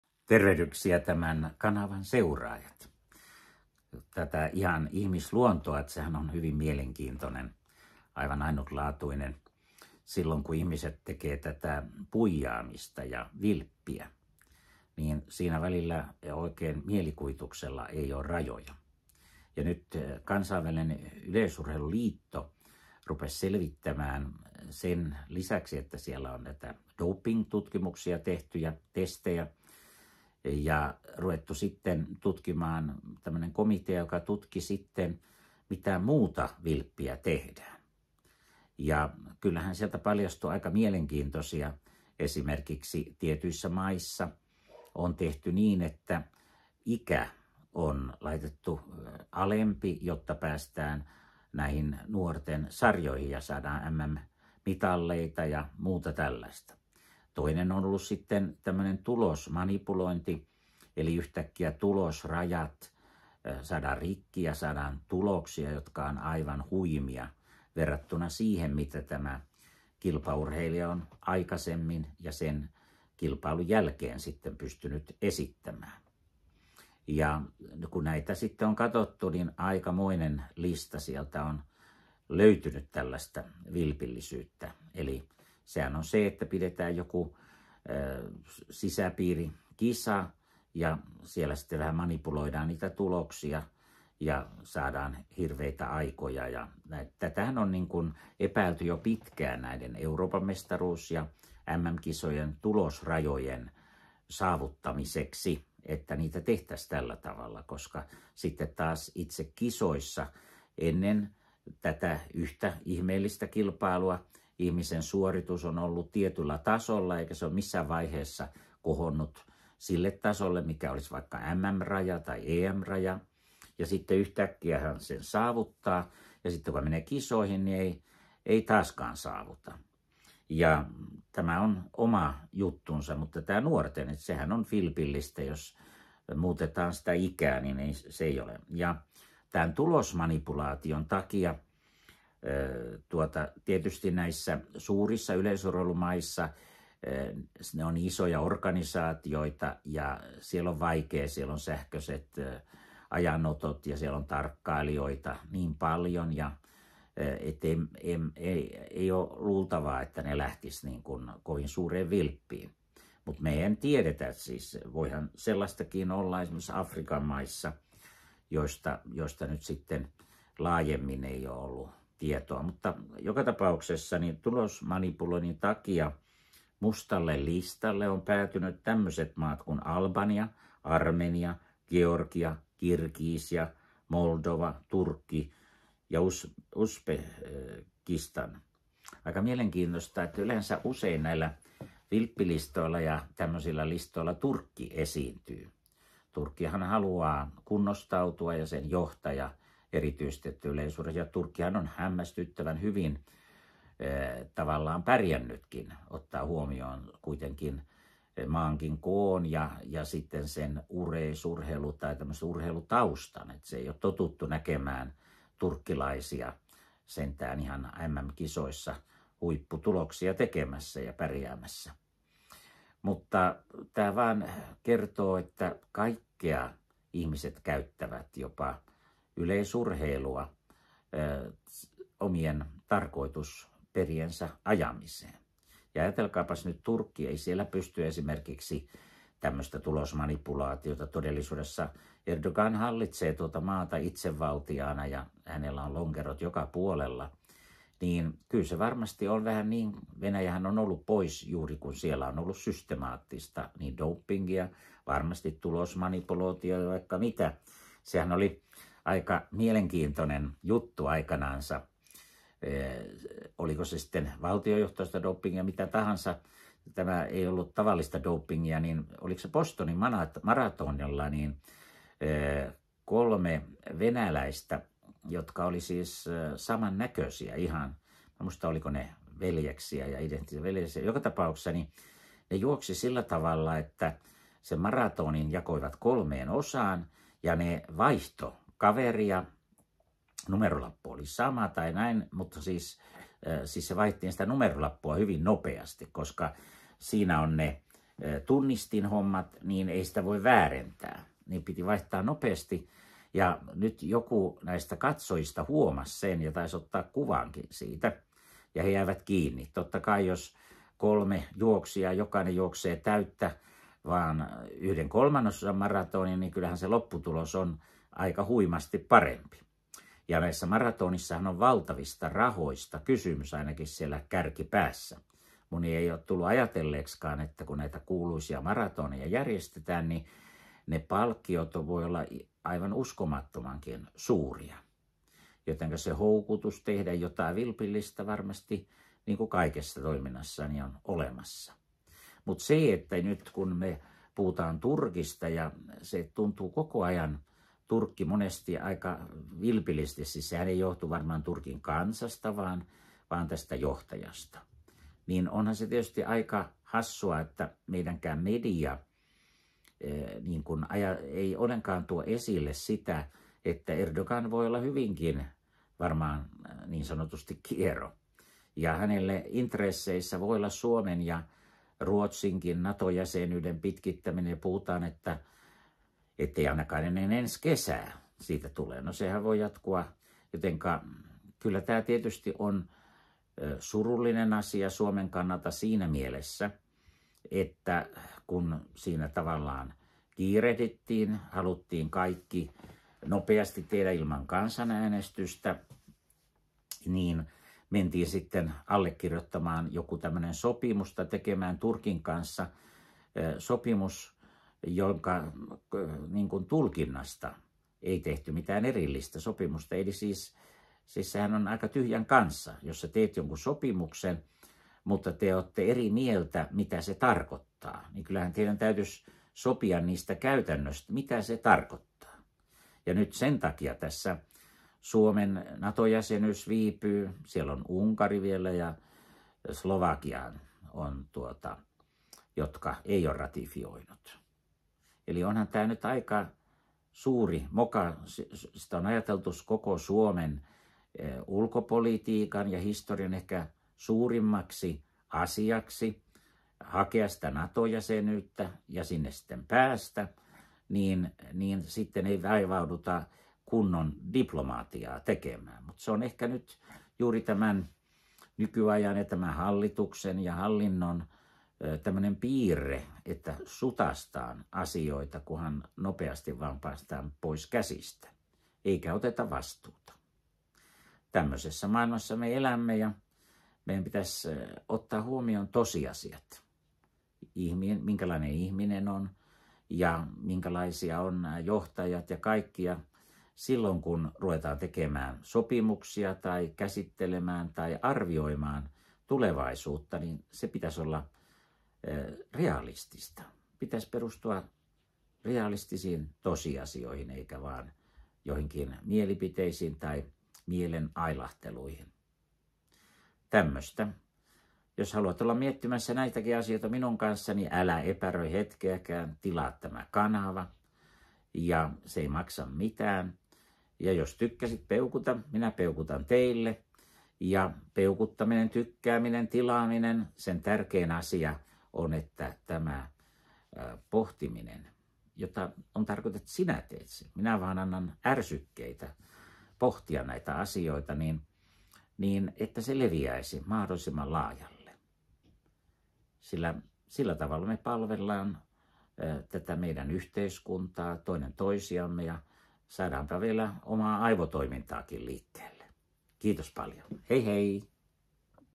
Urheiluhuijaus? Ulkopoliittinen Instituutti Mika Aaltola Tero Pitkämäki Topi Raitanen Lasse Viren
Kansainvälinen Yleisurheiluliitto tulosmanipulointi ja ikärajojen huijaus tutkimus. Huijareiden ja huijari maiden lista on pitkä. Kauan on epäilty Kiinaa tulosmanipuloinnista. Miksi Kiina ei ole listalla?